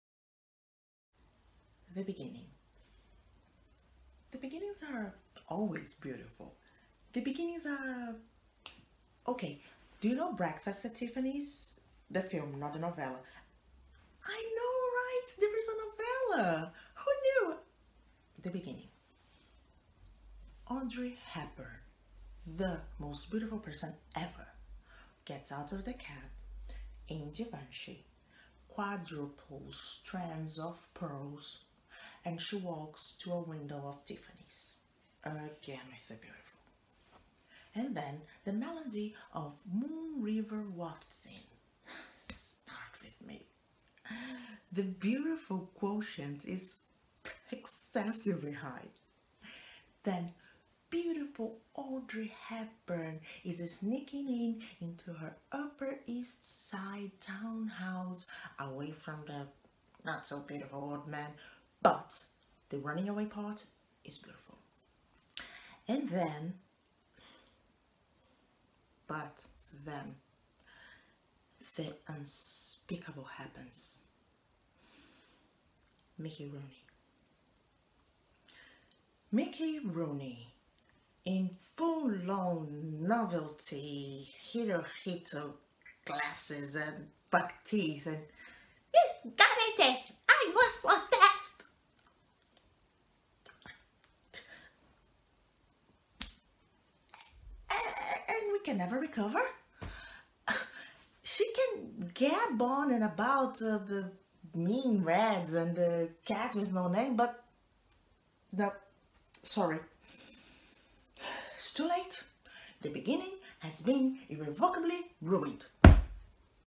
The Little Dog Laughed by Douglas Carter Beane
Brittle lesbian Diane, a top Hollywood agent, wants to make a movie of a hit New York play about male lovers. She's also desperate for her fast-rising client, the discreetly gay Mitchell, to star in it.
GENRE: Comedy